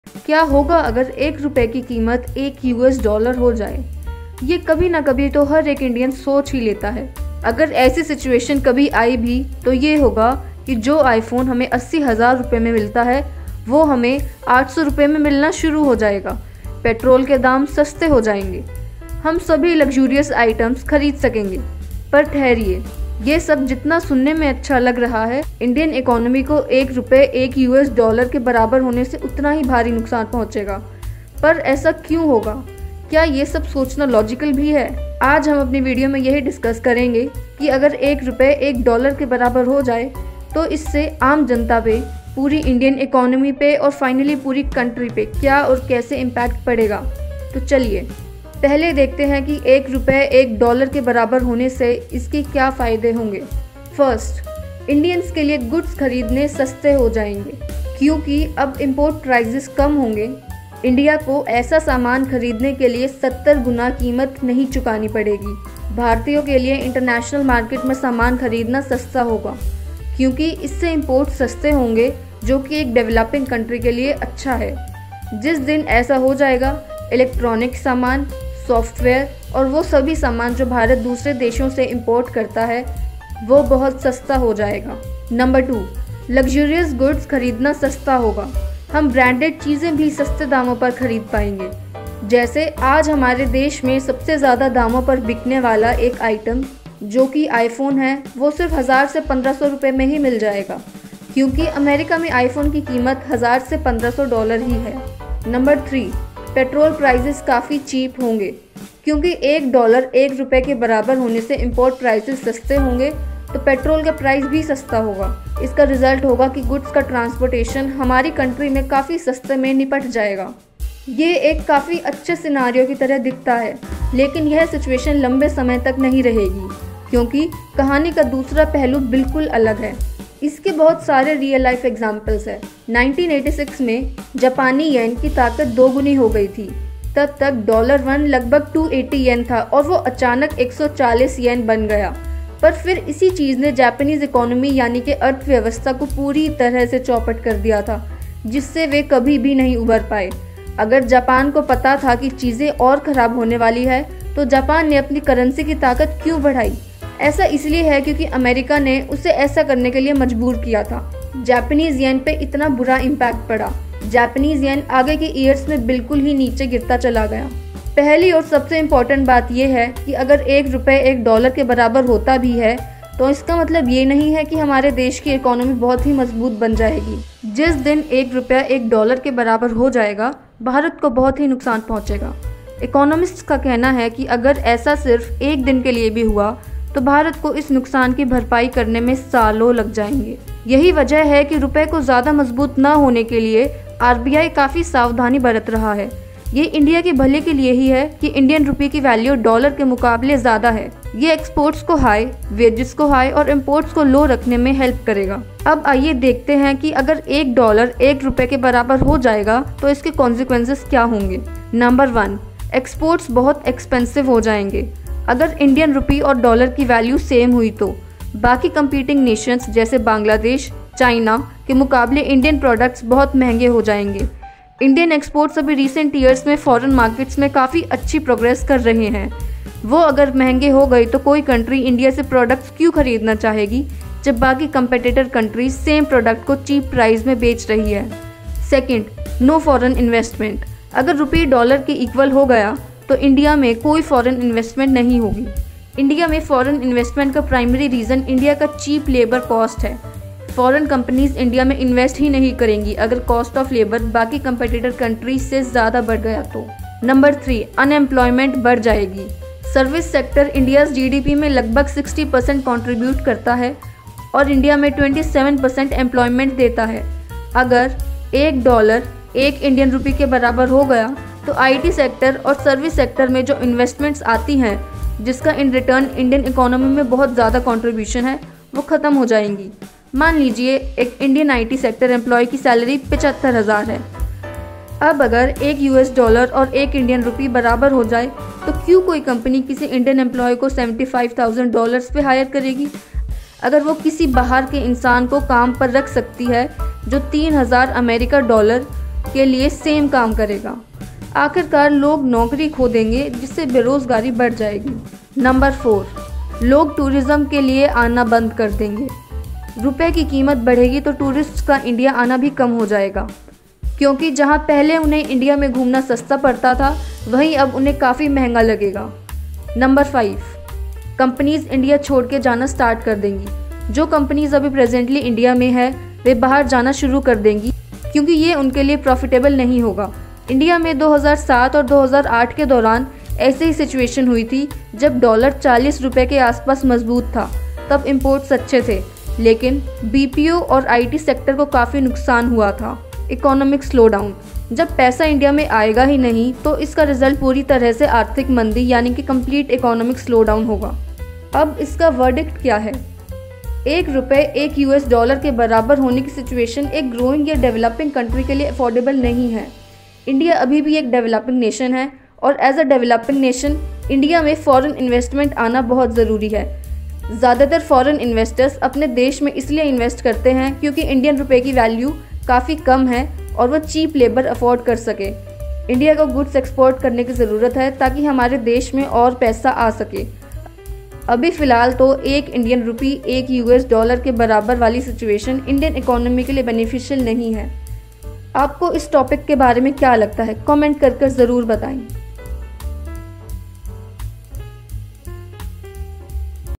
WHAT HAPPENS IF $1=₹1? क्या होगा अगर $1=₹1 हो जाए?
Just imagine if $1 equals ₹1? Will we be able to buy an iPhone and branded items at cheap prices? Will our Indian economy be able to handle a strong Indian currency? Let's check out in this video.